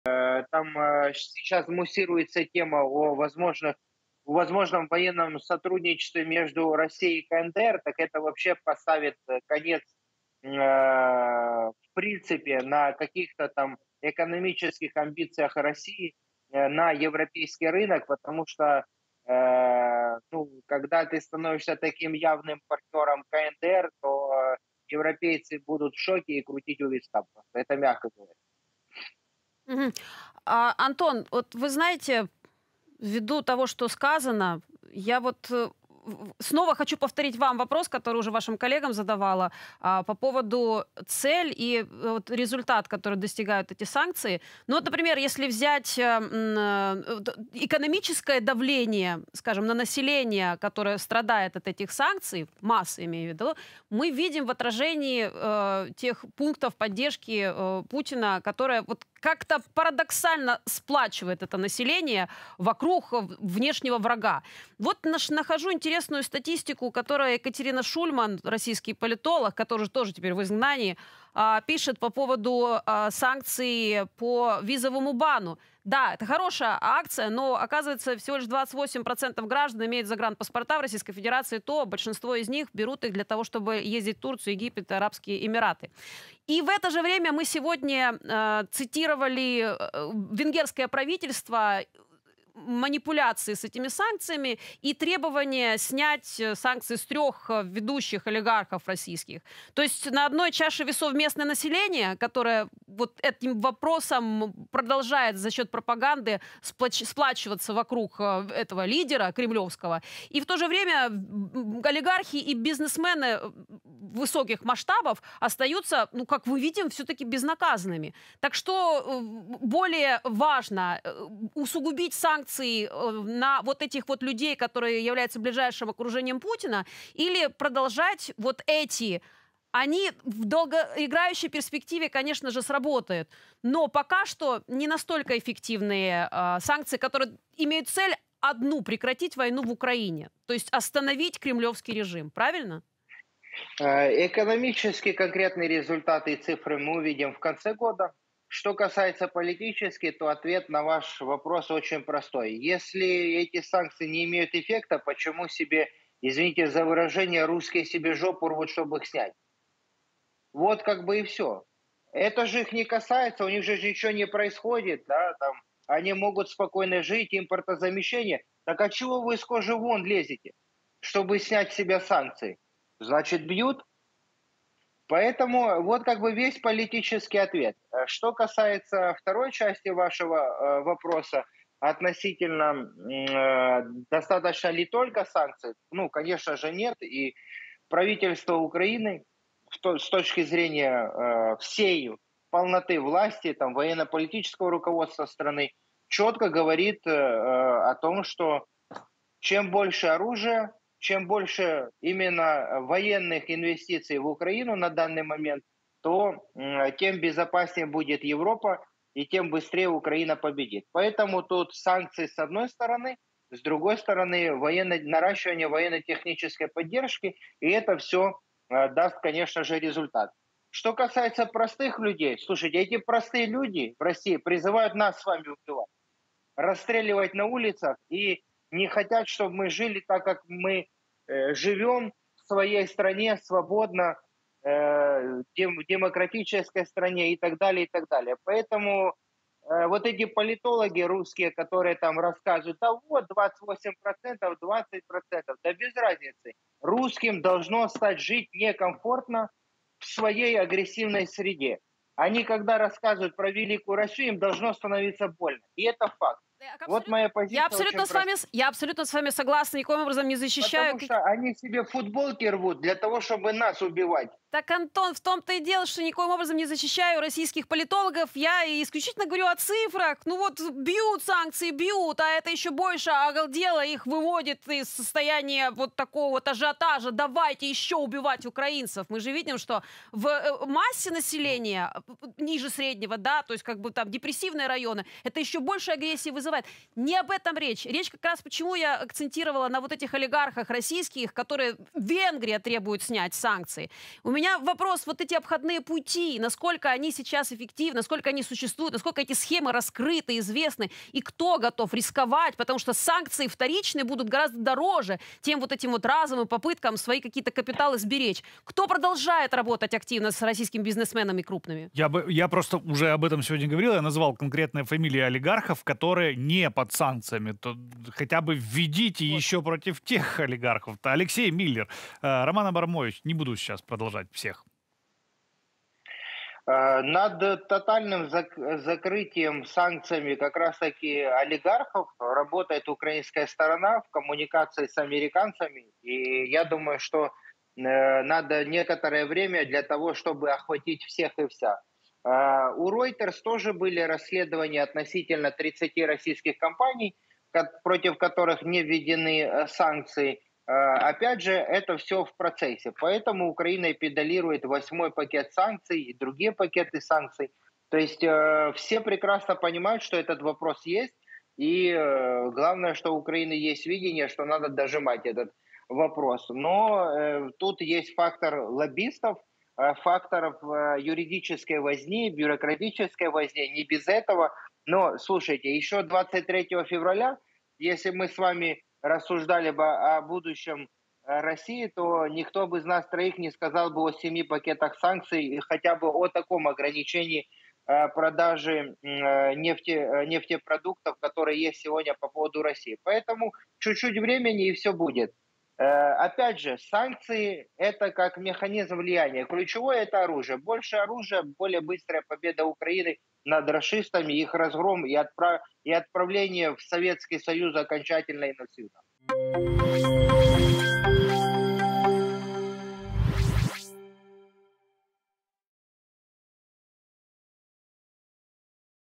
Политолог назвал рецепт победы Украины: больше оружия – безопаснее Европа
UA - Санкції продовжують тиснути на Росію. І що далі, то більше. Але лише одних санкцій для перемоги України замало. Про вплив обмежень на РФ та безпеку Європи висловився політолог-міжнародник Антон Кучухідзе.

RU - Санкции продолжают давить на Россию. И чем дальше, тем больше. Но только одних санкций для победы Украины недостаточно. О влиянии ограничений на РФ и безопасности Европы высказался политолог-международник Антон Кучухидзе.